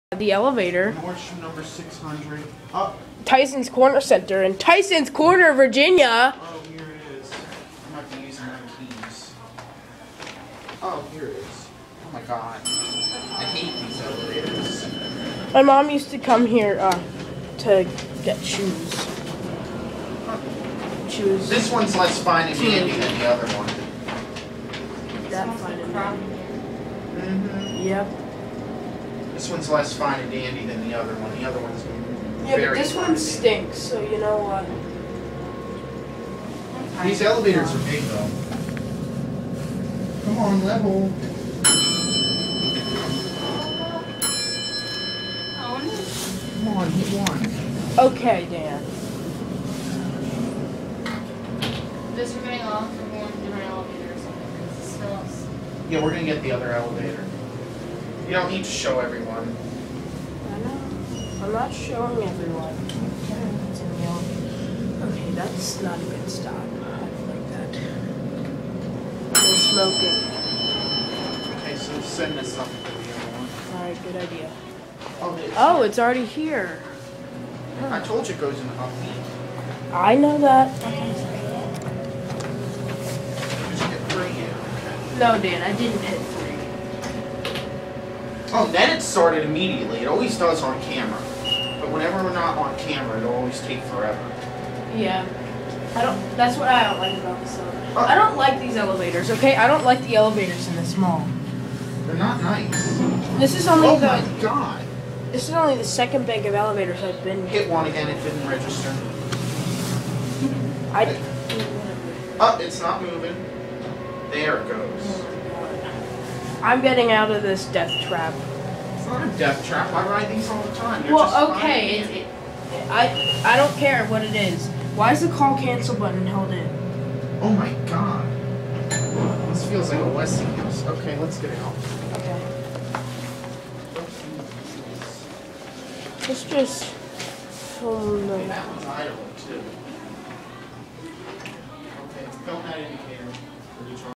Montgomery Traction Elevators @ Nordstrom-Tysons Corner Center, Tysons Corner, VA
filmed w/ dea41396returns

Tech Specs:
Capacity-4000 lbs
Fixtures-Innovation Universal
Floors Served-3
Door Type-Center Opening
Installed-1988